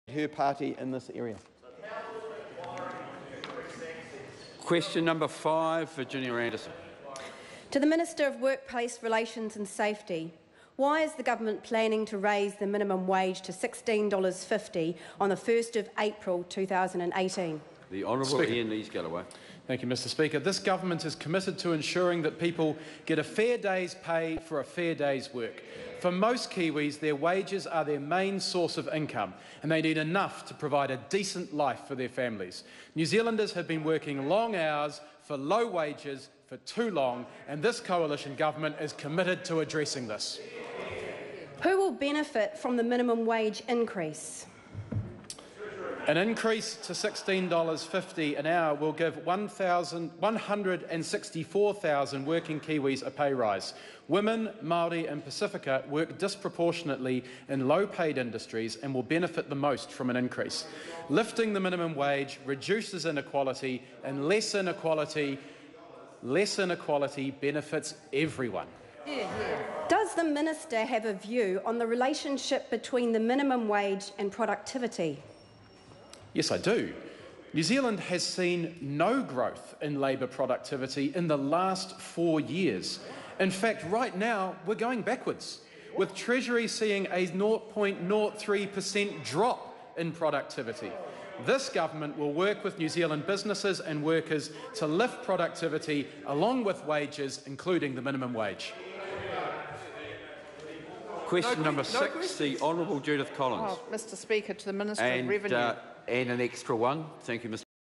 Question 5 - Virginia Andersen to the Minister for Workplace Relations and Safety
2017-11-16 - Question 5 - Virginia Andersen to the Minister for Workplace Relations and Safety

Iain Lees-Galloway,Virginia Andersen